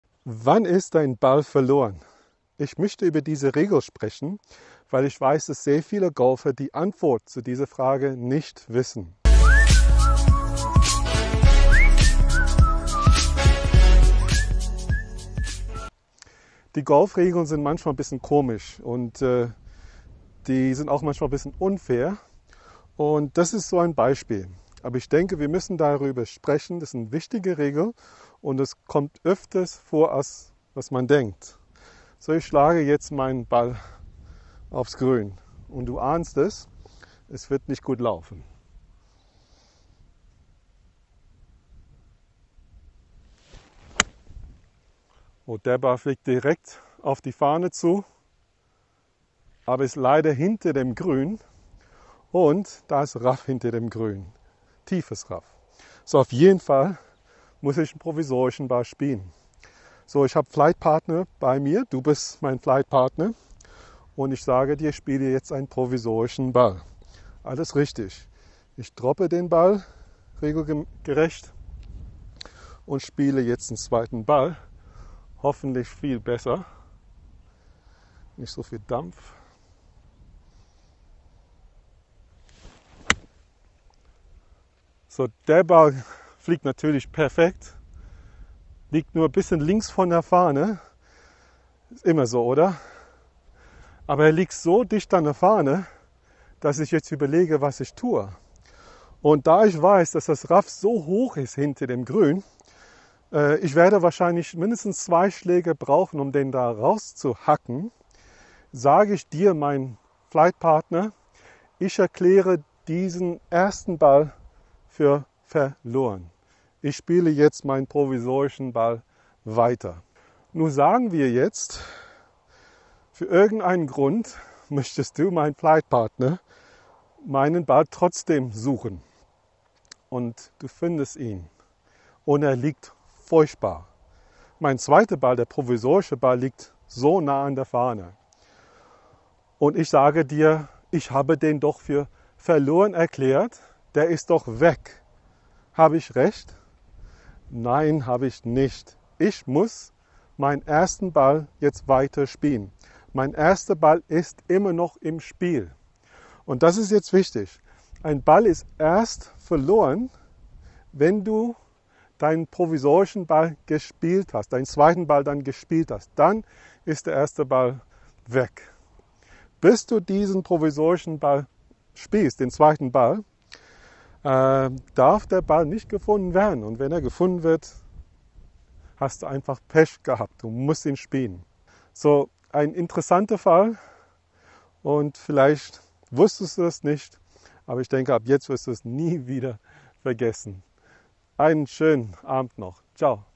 Ist ein Golfball verloren, nachdem er für verloren erklärt wurde?
Wir haben es alle getan: Wir sagen unseren Flugpartnern, dass wir unseren ersten Ball für verloren erklären und stattdessen den provisorischen Ball spielen werden.
Aber ist das richtig und zu welchem ​​Zeitpunkt ist ein Ball tatsächlich verloren?

MÖCHTEST DU EINE GOLFSTUNDE MIT MIR IM GC SCHLOSS LÜTETSBURG BUCHEN?

ÜBERNACHTUNG IM BAUMHAUS!